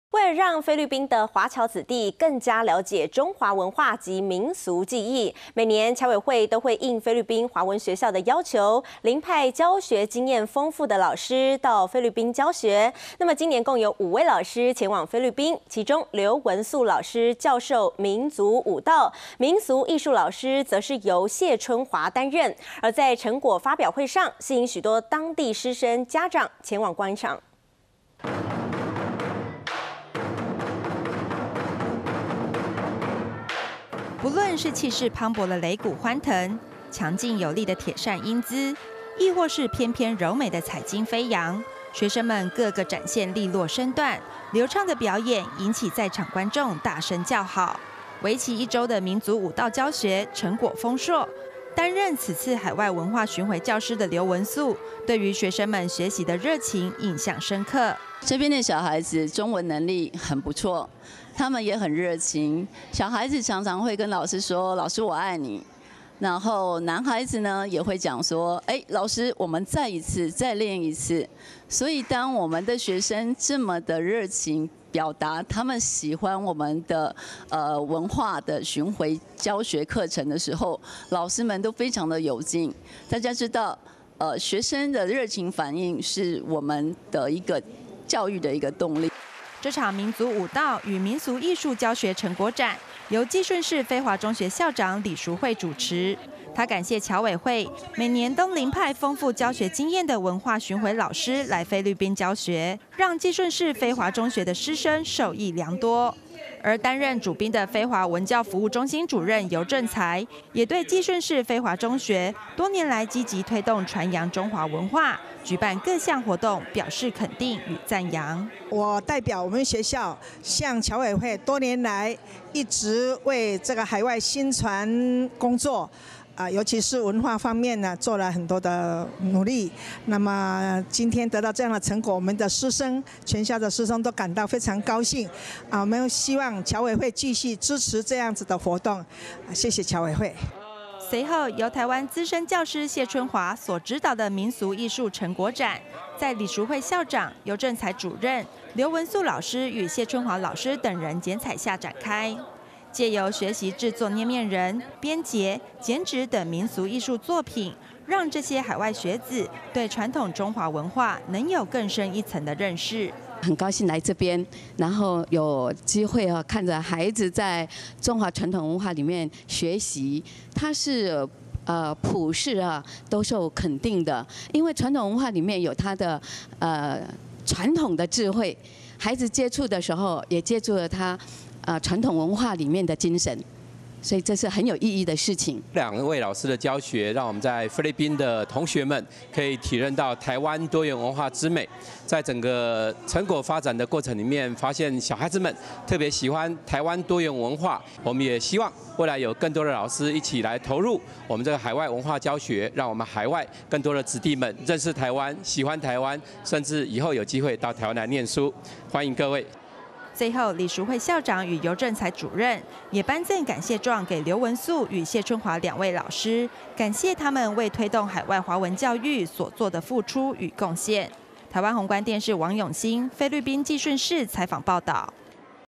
文化志工海外教學 計順菲華中學成果展—宏觀僑社新聞
為了讓菲律賓的華僑子弟更了解中華文化及民俗技藝，每年僑委會都會應菲律賓華文學校的要求，遴派教學經驗豐富的老師，到菲律賓教學；今年共有5位老師前往菲律賓，其中劉文素老師教授民族舞蹈、民俗藝術老師則是謝春華擔任，而在成果發表會上，吸引許多當地師生、家長前往觀賞。

臺灣宏觀電視 王永鑫
菲律賓計順市 採訪報導

➨更多宏觀影片：
《新聞類》